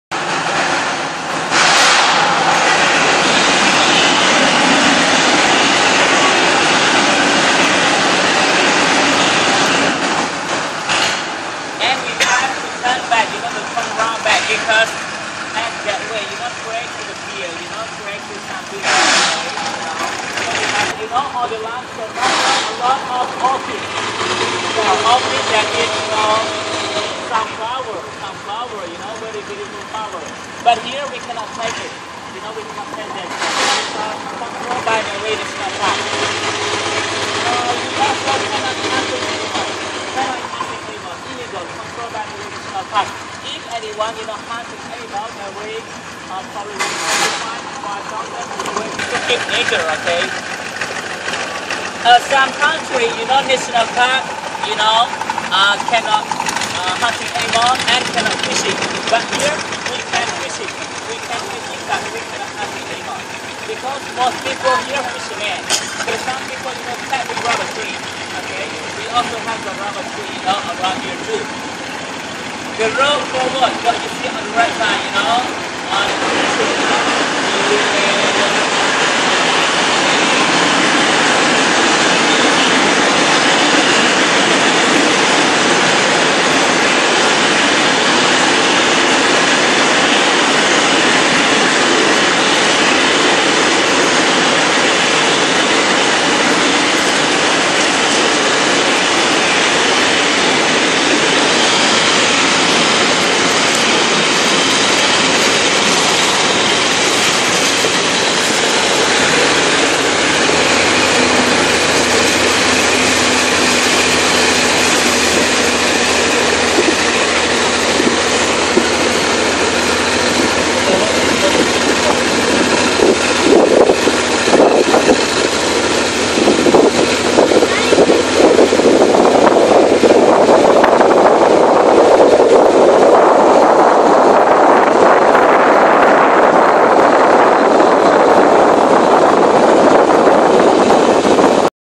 Tham Lod at Phangnga Bay is unseen thailand.
The trip highlights local culture, beauty of Phang Nga Bay, fertilities of mangrove forests, etc.